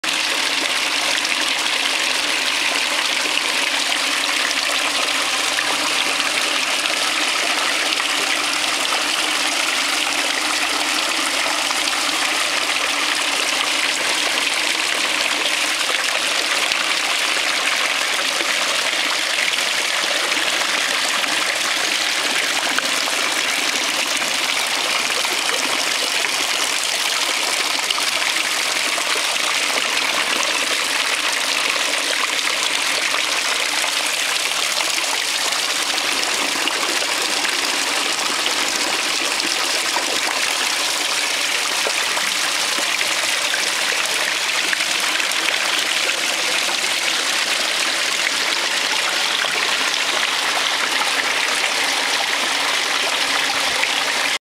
video les couleurs , fontaine
Fontaine en hommage pour tous les artistes du MONDE art plastique  prix 20 000 £